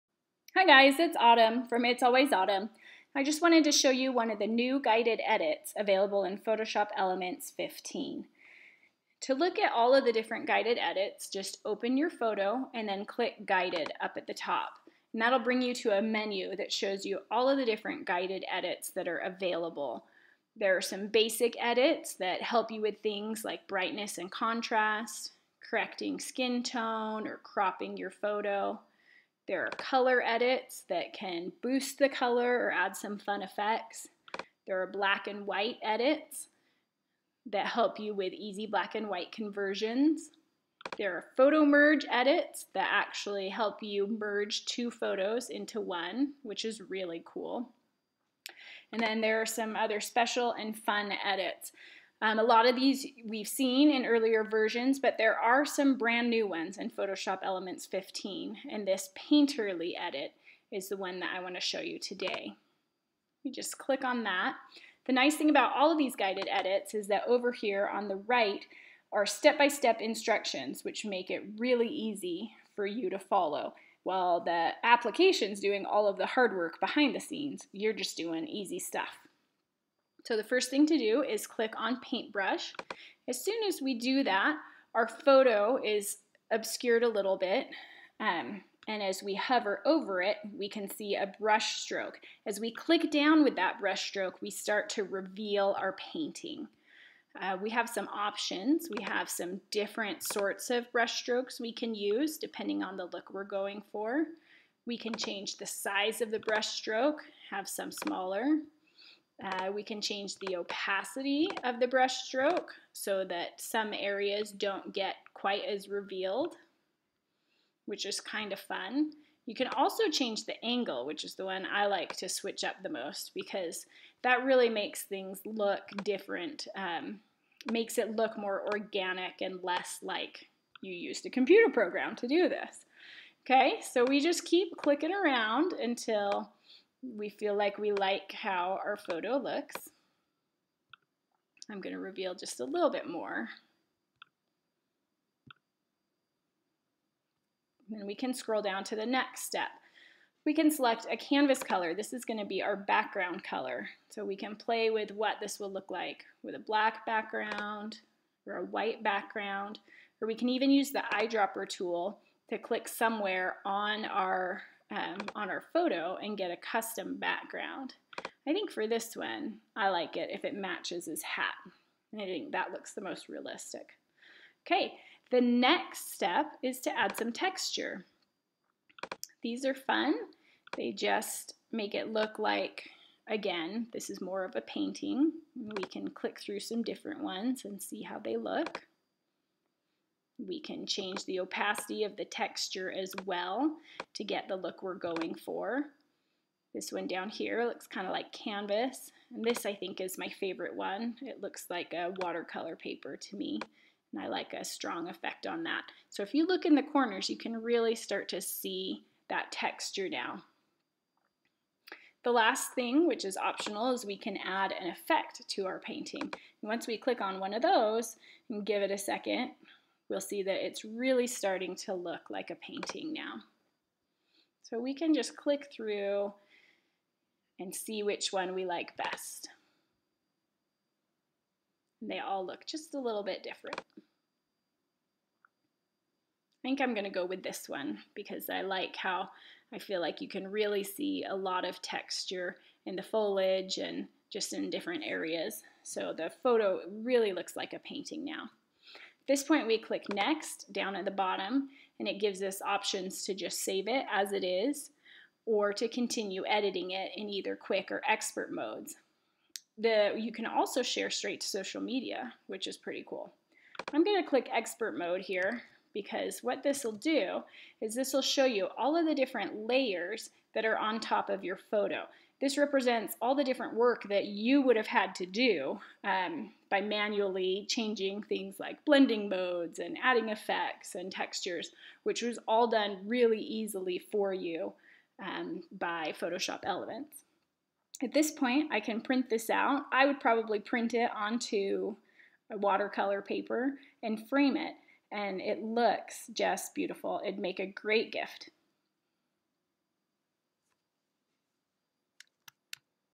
Bring Memories to Life with Painterly feature with It's Always Autumn
Bring memories to life with all-new Photoshop Elements 15. Join one of our new Photoshop Elements Creators, Its Always Autumn, as she walks you through her favorite new features and the Painterly guided edit